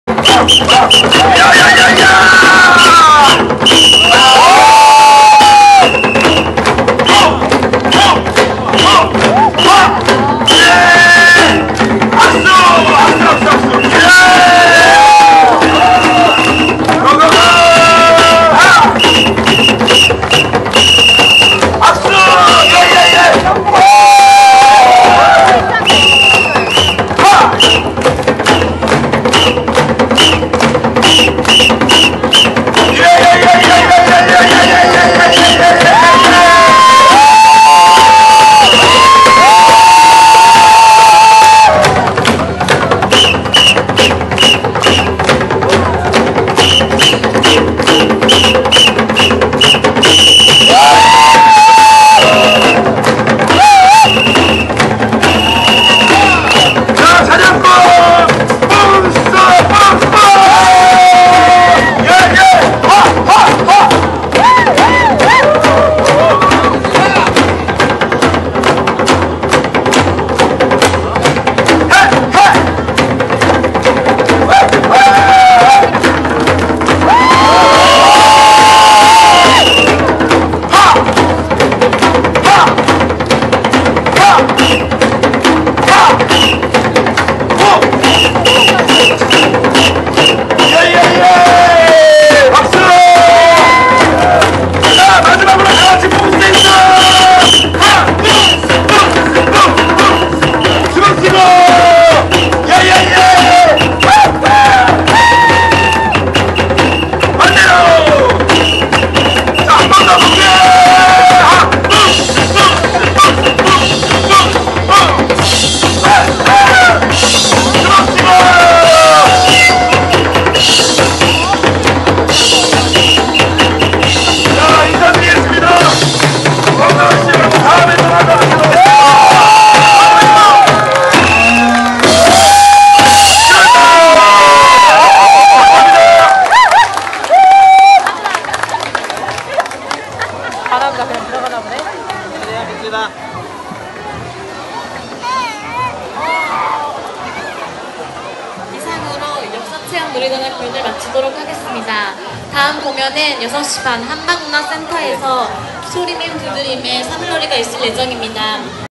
한옥마을에서 본 원시인들의 재미있는 댄스 Jeonju Hanok village
Jeonju city. KOREA
전주한옥마을 Jeonju Hanok village 全州市韓屋村